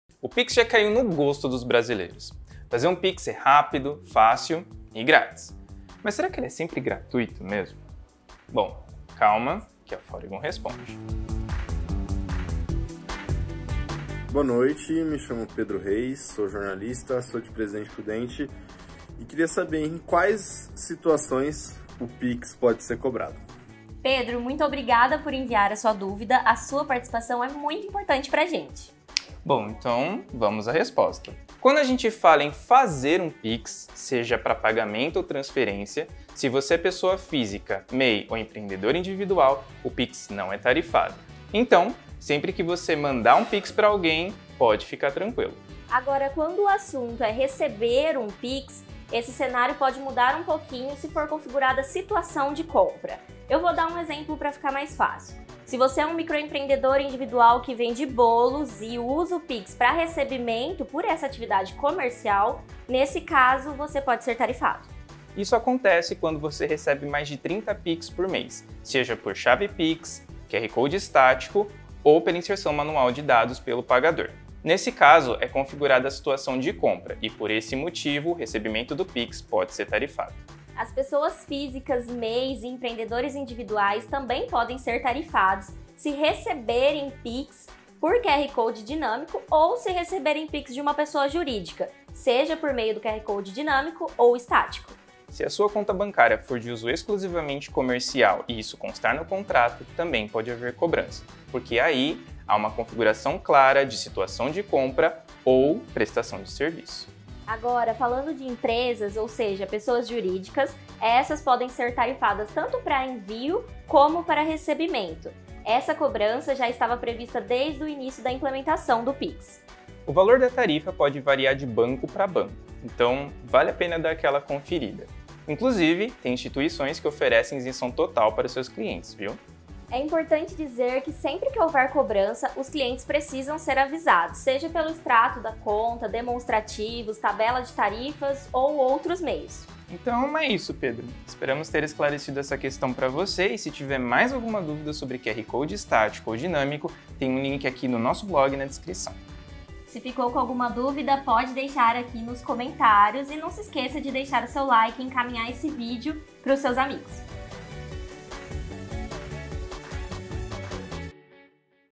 PIX PODE SER COBRADO? - Foregon.com
PIX PODE SER COBRADO?

O PIX já caiu no gosto dos brasileiros. Fazer um PIX é fácil, rápido e grátis.
Mas ele é sempre gratuito mesmo? Calma, que a Foregon Responde.

O PIX não cobra taxa de transferência para pessoas físicas, mas pessoas jurídicas estão sujeitas a taxas. Entenda melhor neste vídeo.